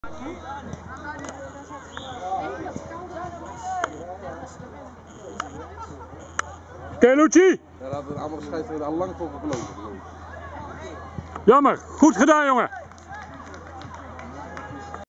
Luchiano Nijhuis (Rijen B4) dolt tegenstander Desk B4
Rijen B4 heeft uit tegen Desk B4 weinig in te brengen en verliest met 10-1. Luchiano Nijhuis (14) lukt het toch regelmatig zijn directe tegenstander tot wanhoop te brengen. Jammer genoeg komt hier geen goal uit voort. Het gebeurde op zaterdag 22 september 2012.